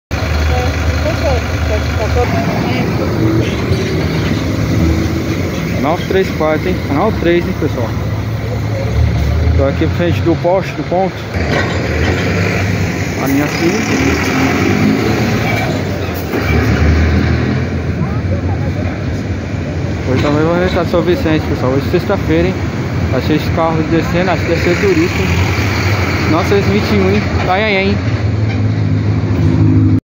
Rua Padre Anchieta São Vicente SP ônibus Linha 934 E Linha 05 Br Mobilidade Marcopolo Torino S 🤠
Linha 934 Canal 3 Central
Linha 05 Canal 1
Linha 921 Canal 1 Itanhaém